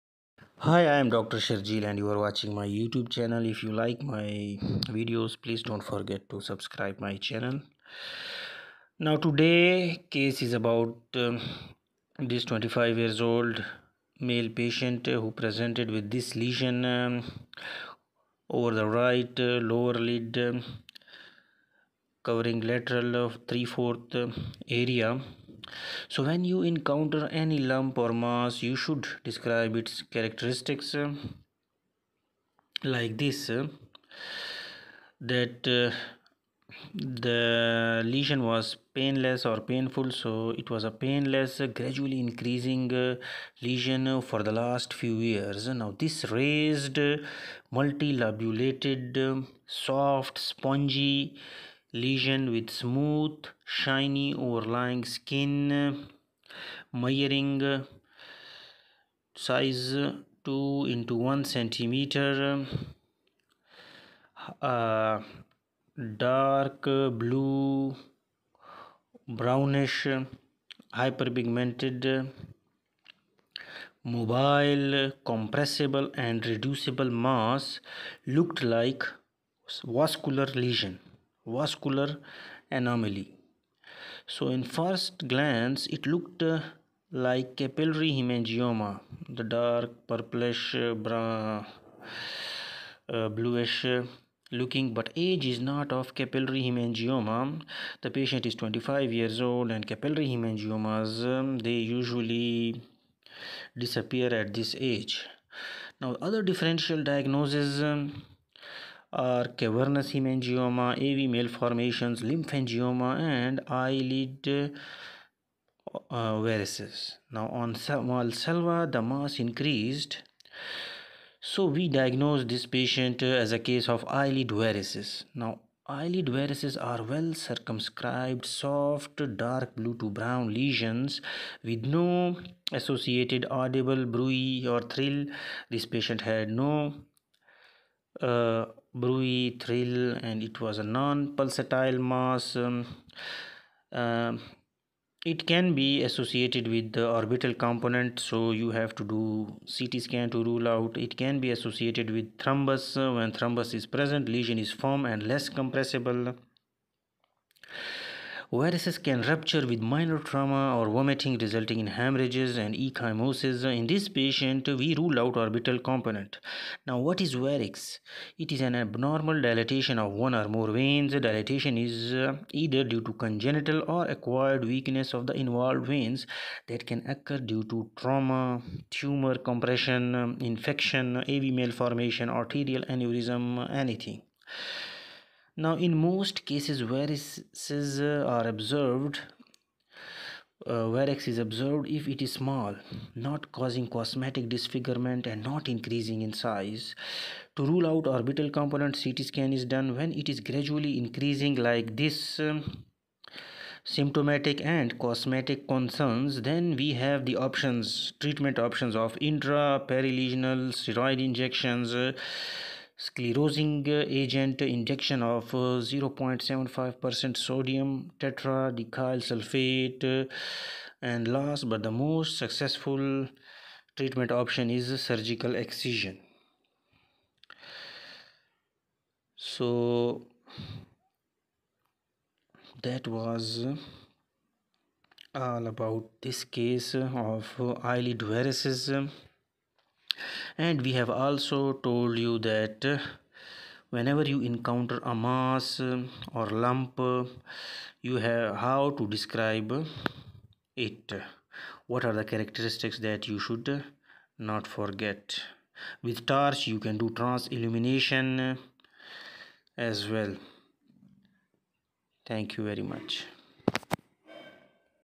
Eyelid varices
In this video I will discuss differentials of vascular growth on eyelid and also briefly tell you how to describe a lump and what are the treatment options for eyelid varix